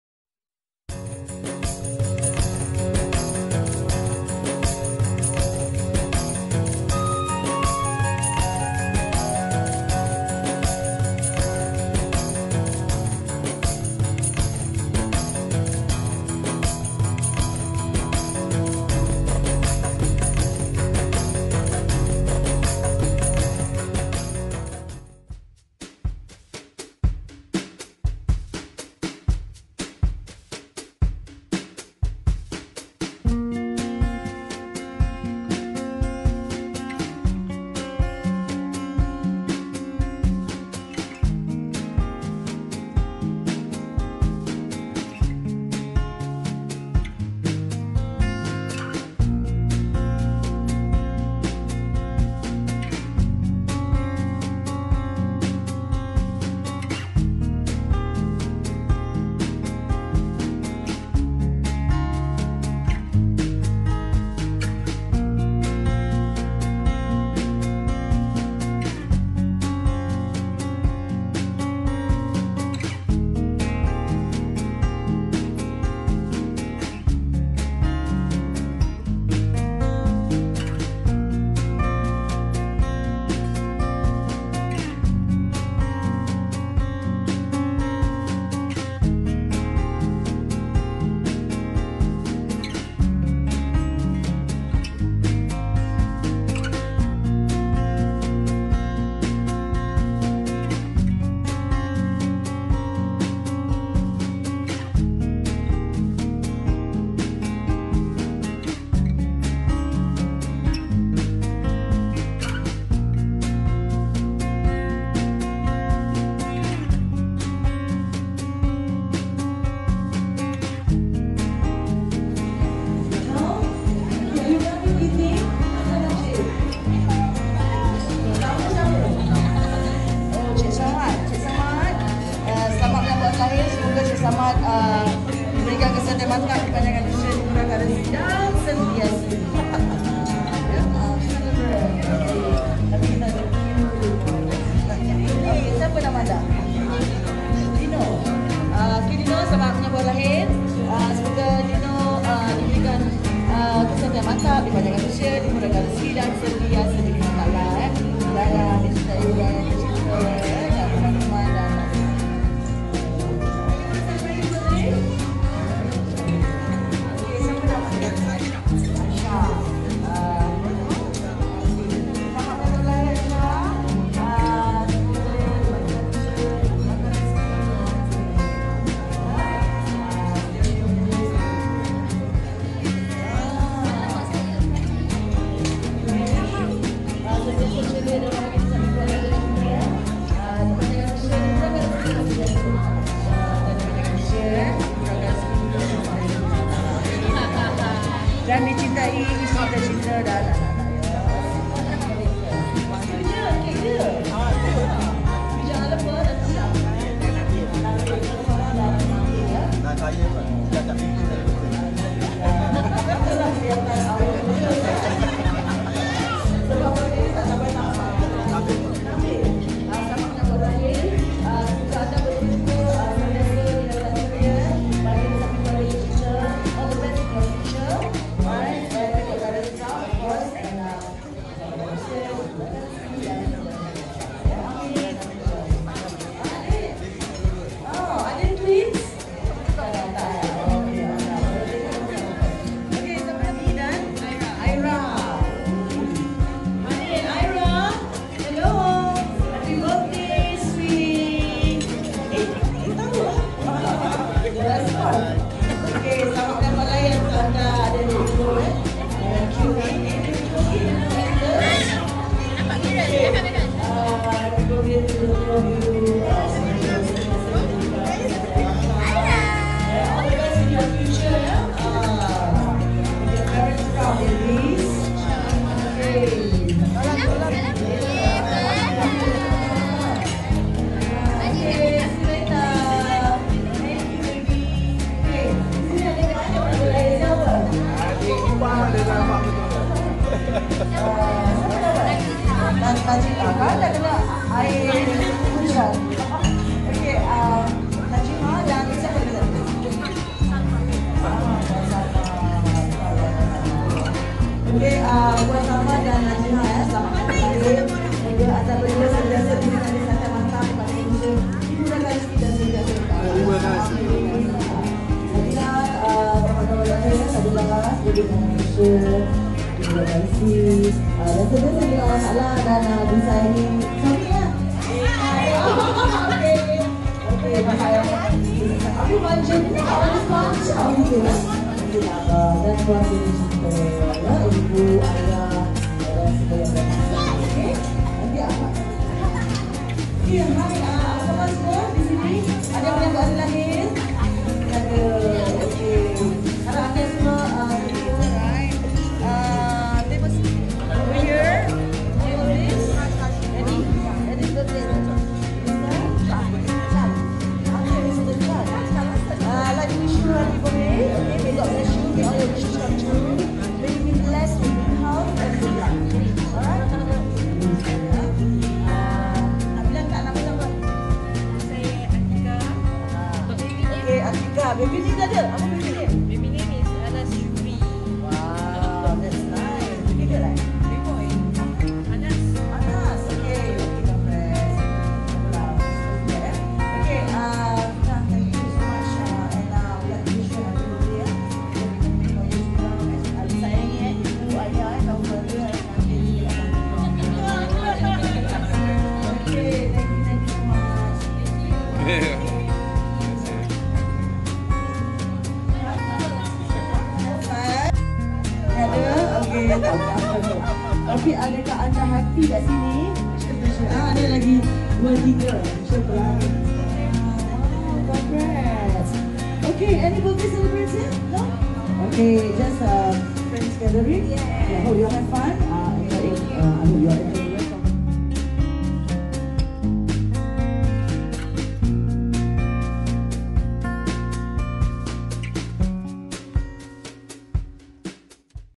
THE ROYAL PALM RESTAURANT ORCHID COUNTRY CLUB. HALAL CERTIFIED BY MUIS. 17TH OCTOBER 2018.
EXCELLENT FOOD AND GREAT LIVE ENTERTAINMENT BY THE BAND BEAUTY AND THE BEAST EVERY TUESDAY TO SUNDAY.